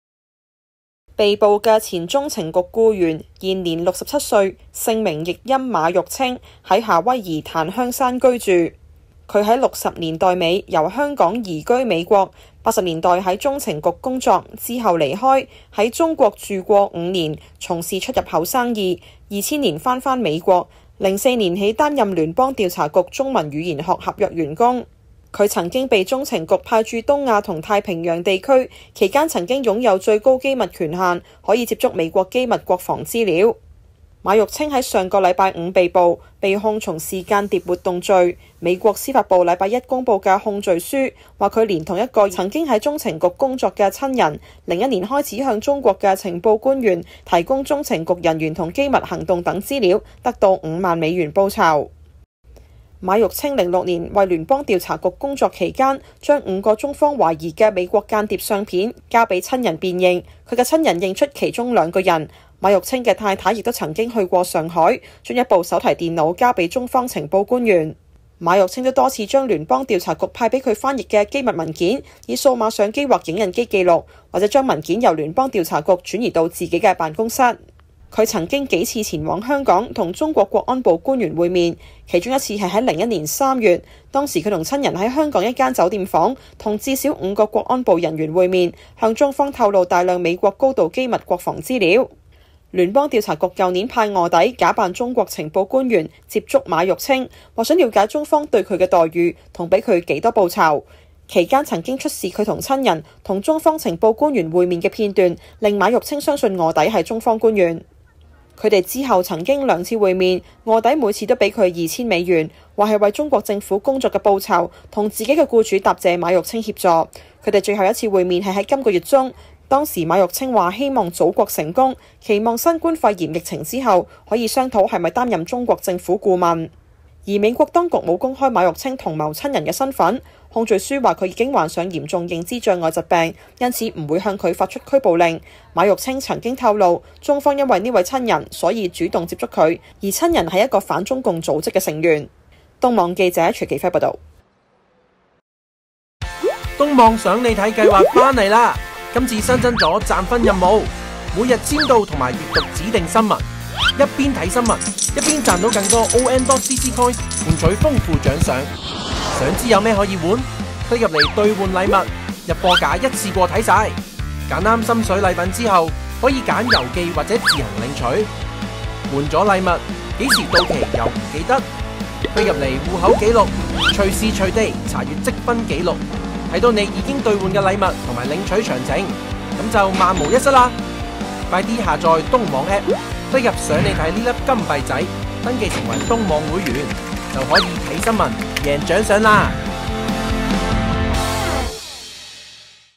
【on.cc東網】港產CIA前僱員在檀香山落網 被控為中國從事間諜活動
再有懷疑「港產雙面諜」在美國被捕！美國司法部周一（17日）公布，由香港移居美國多年的中央情報局（CIA）前僱員馬玉清（Alexander Yuk Ching Ma，音譯）因涉嫌早年向中國出售美國機密，於上周五（14日）在夏威夷檀香山落網，並被控從事間諜活動罪名。夏威夷區檢察官普賴斯（Kenji Price）形容，馬玉清的非法行為背叛美國，又指「那些向中國提供機密資料人士無法逃脫法律長臂」。
【港產CIA前僱員在檀香山落網　被控為中國從事間諜活動】